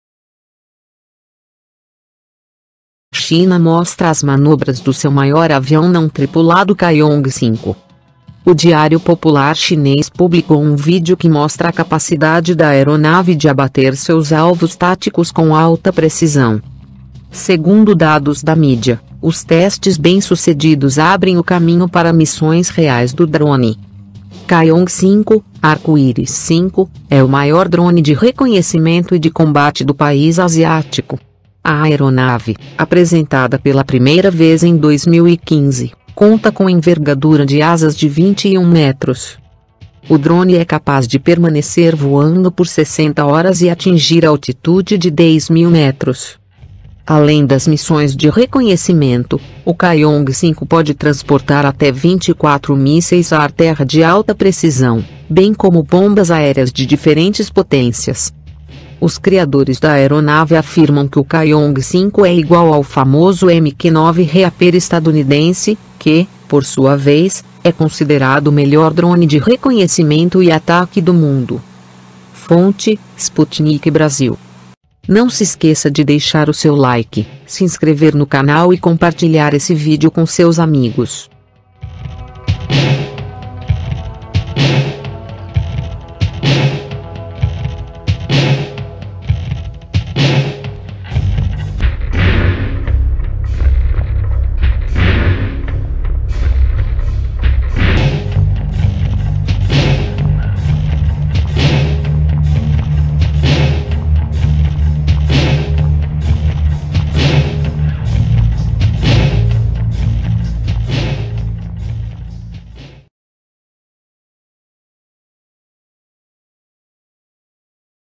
China Cria Maior Drone de Combate e mantém em Igualada aos EUA
A China mostra as manobras do seu maior avião não tripulado Caihong 5.

O Diário Popular chinês publicou um vídeo que mostra a capacidade da aeronave de abater seus alvos táticos com alta precisão.

Segundo dados da mídia, os testes bem-sucedidos abrem o caminho para missões reais do drone.

Caihong 5 (Arco-íris 5) é o maior drone de reconhecimento e de combate do país asiático. A aeronave, apresentada pela primeira vez em 2015, conta com envergadura de asas de 21 metros.

O drone é capaz de permanecer voando por 60 horas e atingir altitude de 10.000 metros.

?Além das missões de reconhecimento, o Caihong 5 pode transportar até 24 mísseis ar-terra de alta precisão, bem como bombas aéreas de diferentes potências.

Os criadores da aeronave afirmam que o Caihong 5 é igual ao famoso MQ-9 Reaper estadunidense, que, por sua vez, é considerado o melhor drone de reconhecimento e ataque do mundo.

☑️ Recomendação Numero 1 para Aprender a Ganhar Dinheiro

China Cria Maior Drone de Combate  e mantem  em  Igualada  aos  EUA